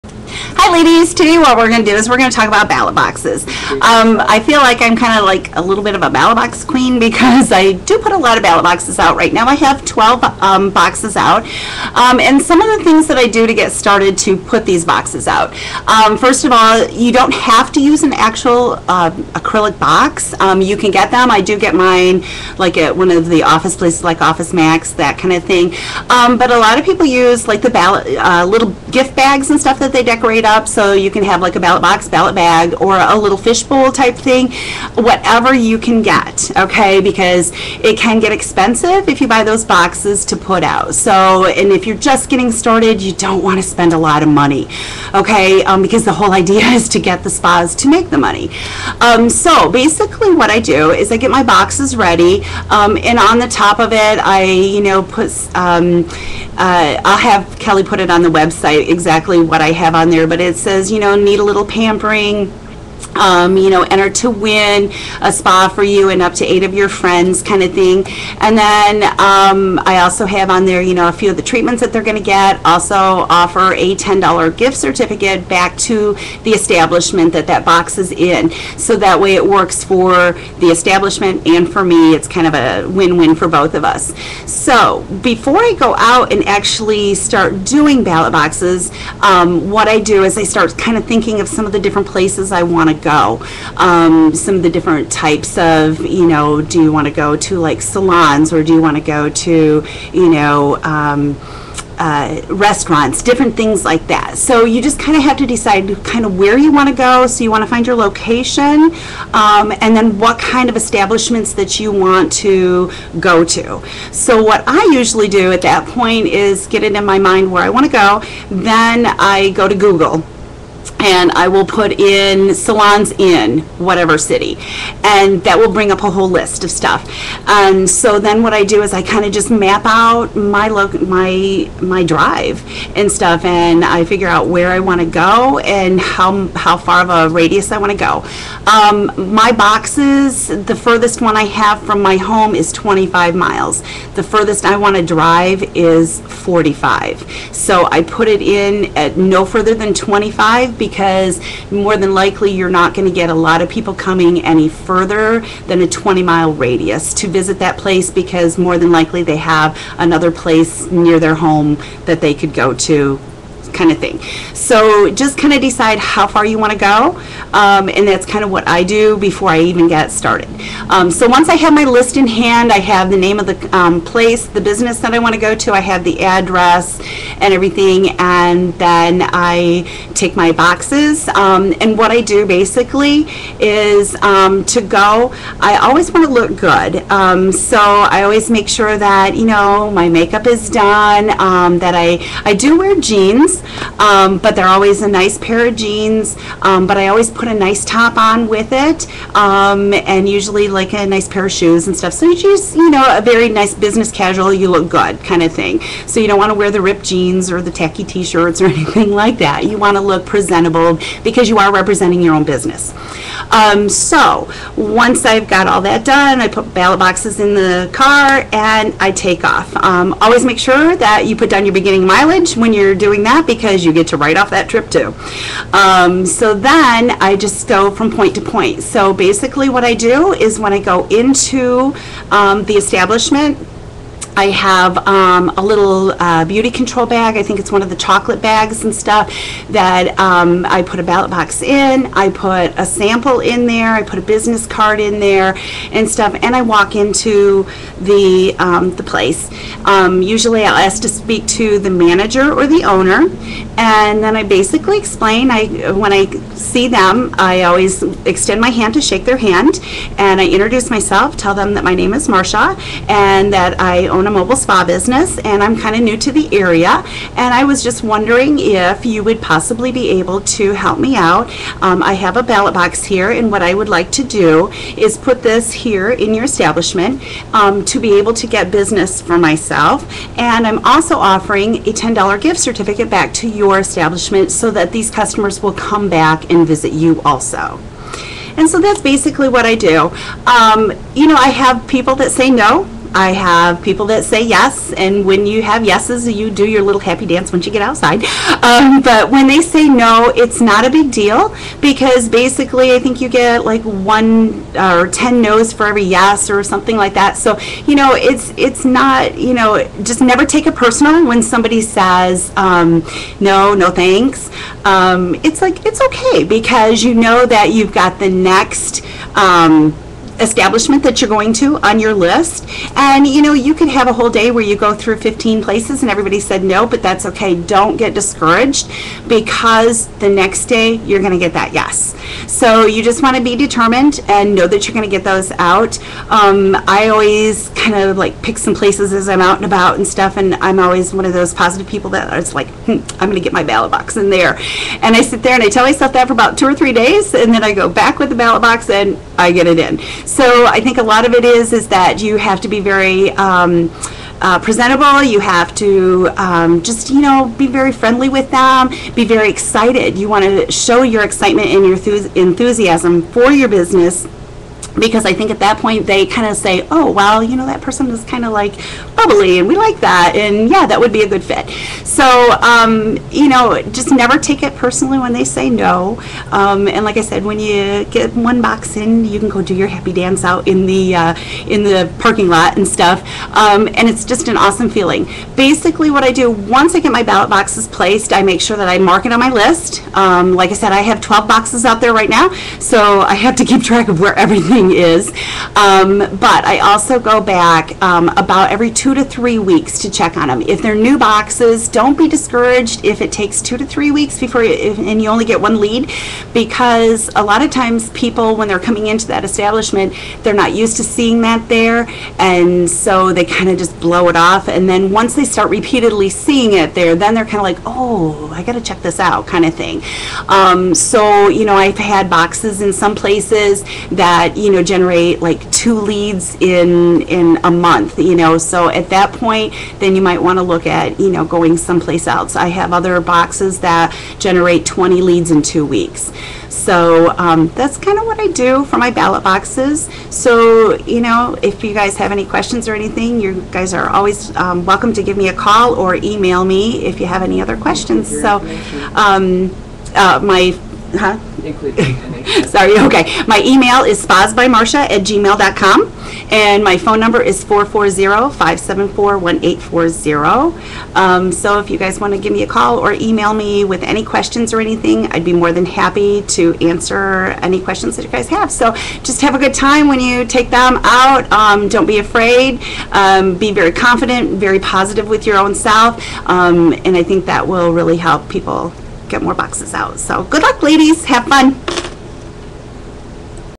Marsha Bosma talks about Ballot Boxes/Bags & How to Get Out Into the Community
kellyatkellyspadivas's Webcam Video from January 26, 2012 06:52 AM